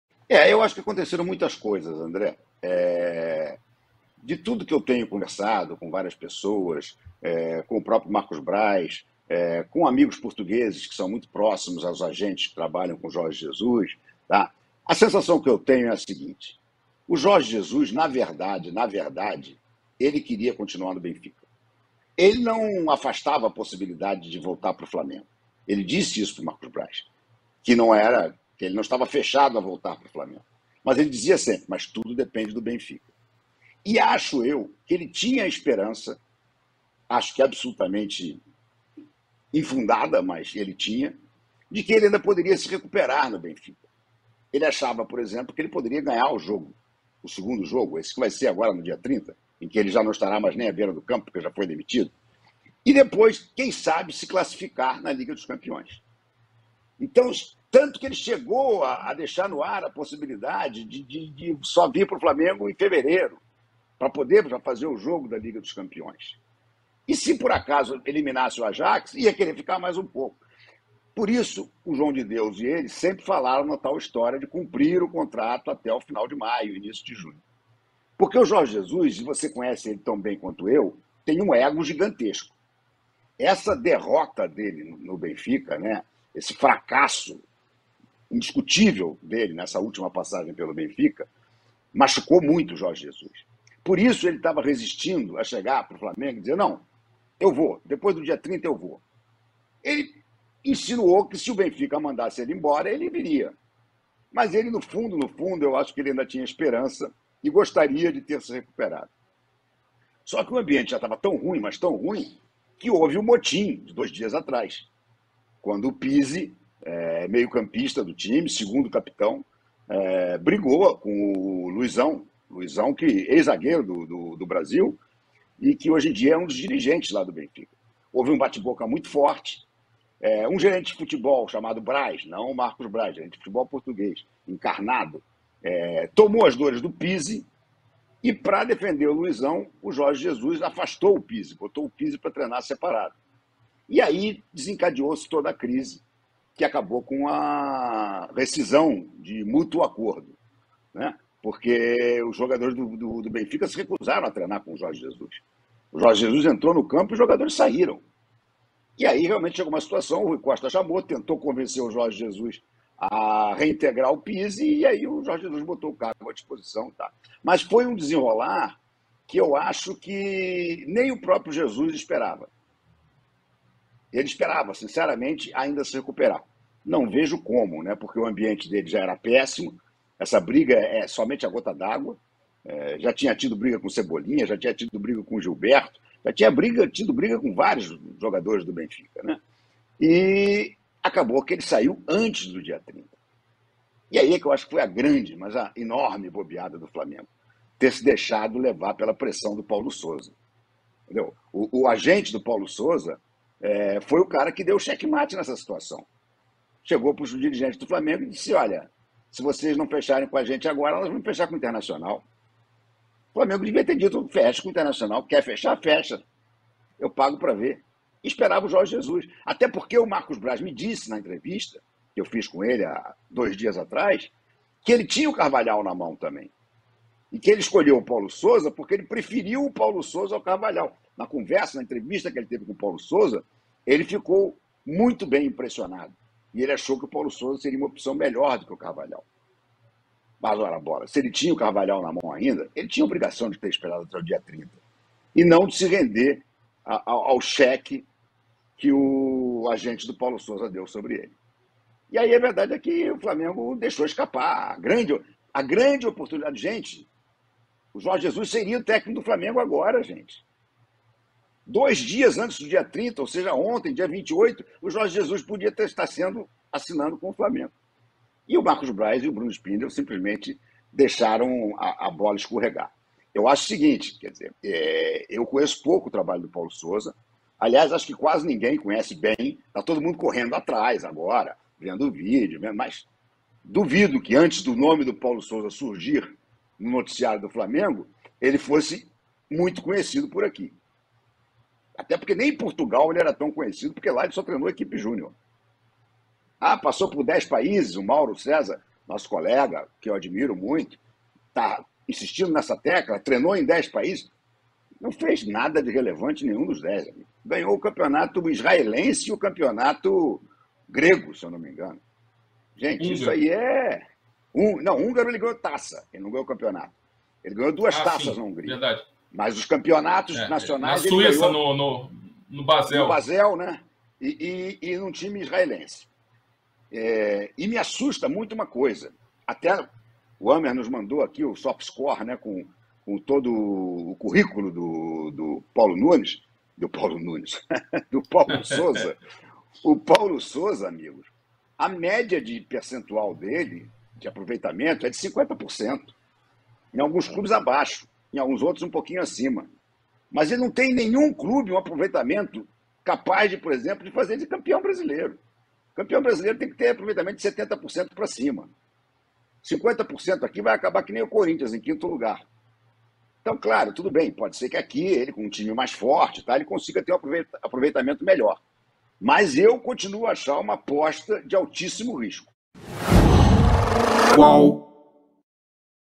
🔴⚫ RENATO MAURÍCIO PRADO: FLAMENGO caiu em XEQUE-MATE do agente de PAULO SOUSA e JORGE JESUS escapou
Na Live do Flamengo UOL desta quarta-feira (29), Renato Maurício Prado disse que o Flamengo não precisaria ter fechado com Paulo Sousa de imediato.